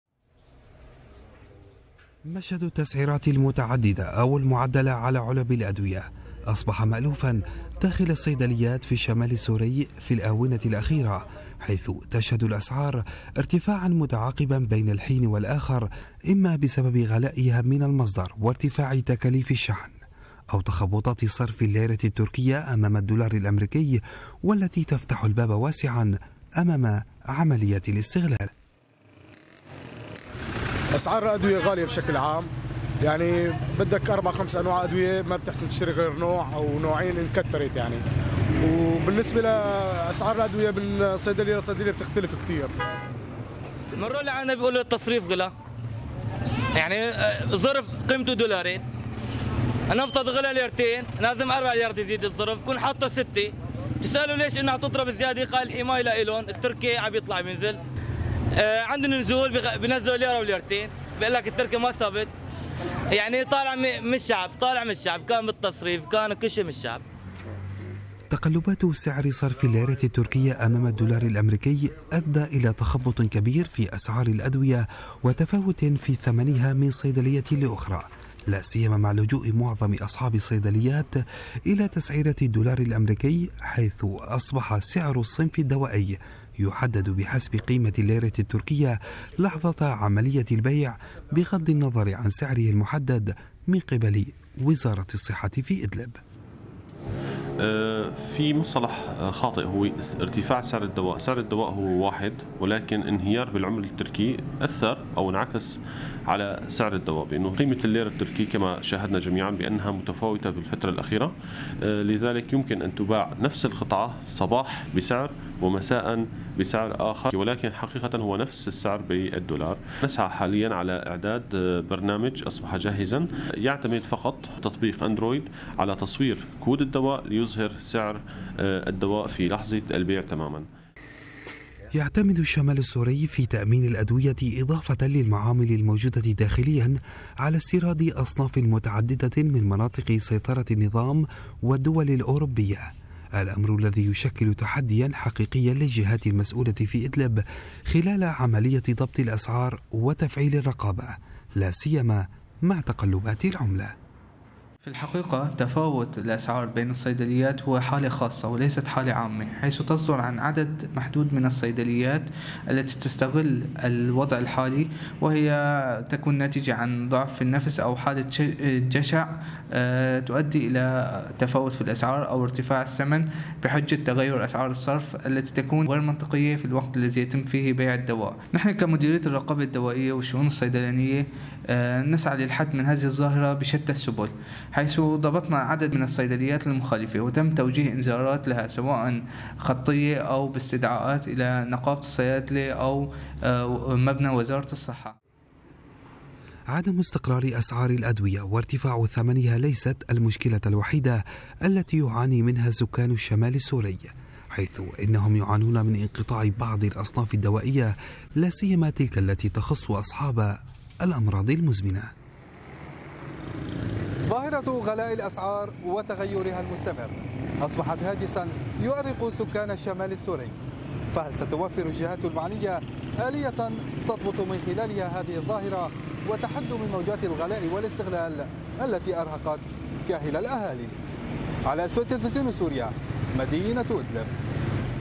أزمة الأدوية في إدلب بين الغلاء والانقطاع
عدمُ استقرارِ أسعارِ الأدوية وارتفاعُ ثمنِها ليست المشكلةَ الوحيدةَ التي يعاني منها سكانُ الشمالِ السوري، حيث إنهم يعانون من انقطاعِ بعضِ الأصنافِ الدوائية لاسيما تلك التي تخصُ أصحابَ الأمراضِ المزمنة
إعداد: علاء السويد